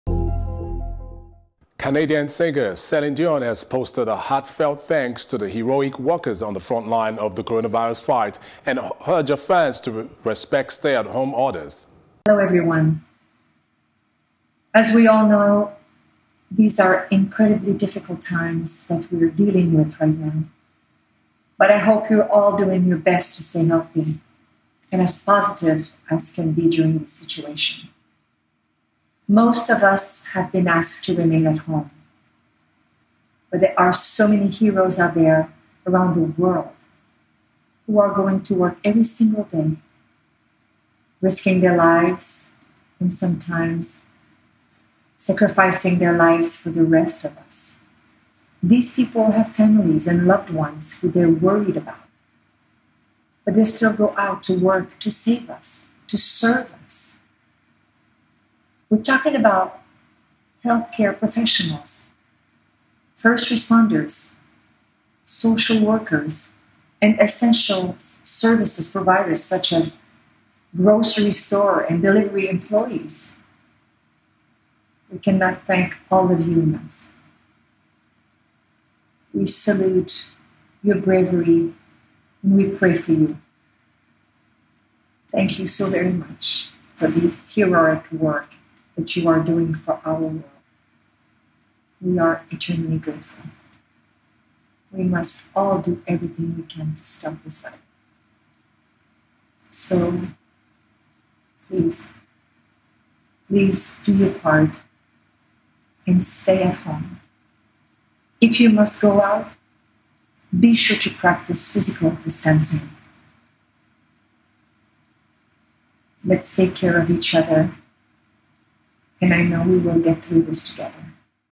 COVID-19: Celine Dion Salutes Front line Workers
Canadian singer Celine Dion has posted a heartfelt thanks to the “heroic” workers on the frontline in the coronavirus fight and urged her fans to respect stay-at-home orders.